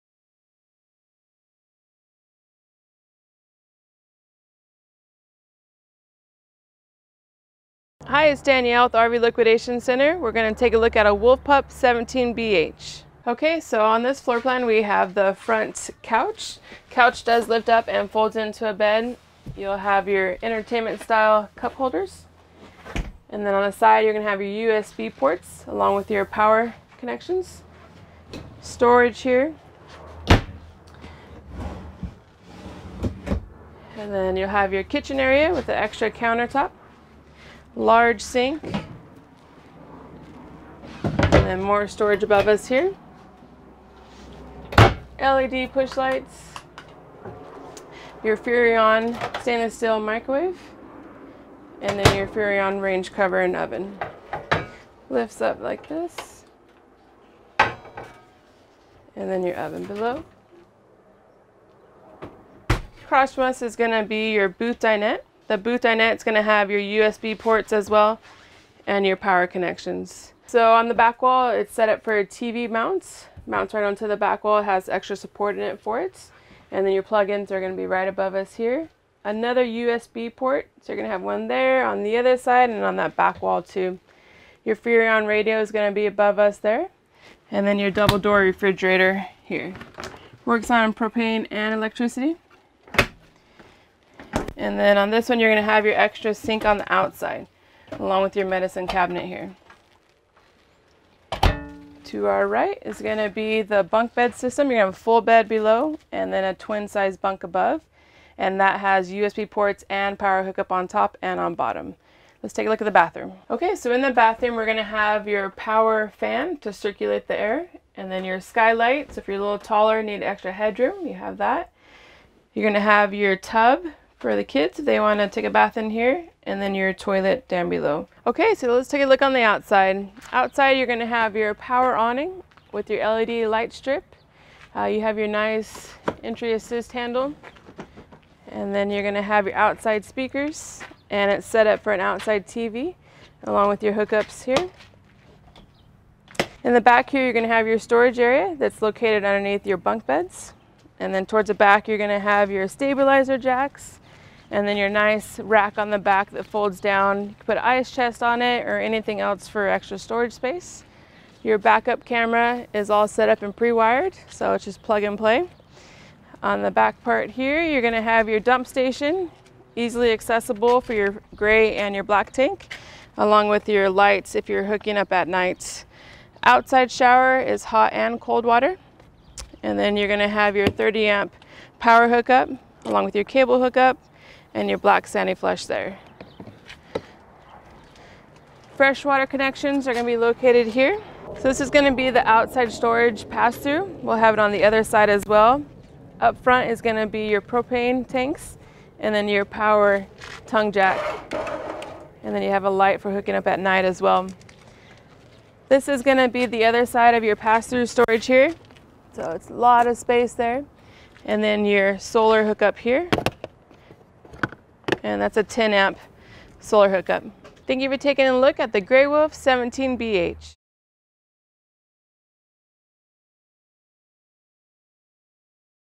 Forest River Cherokee Grey Wolf 17BH Walkthrough
Take a tour of this Forest River Cherokee Grey Wolf 17BH with RV Liquidation Center's, Danielle.

Watch as she walks you through each highlight of the unit.  If you would like to see more - come give us a visit!  We are located on Shaw Ave and Hwy 168 in Clovis!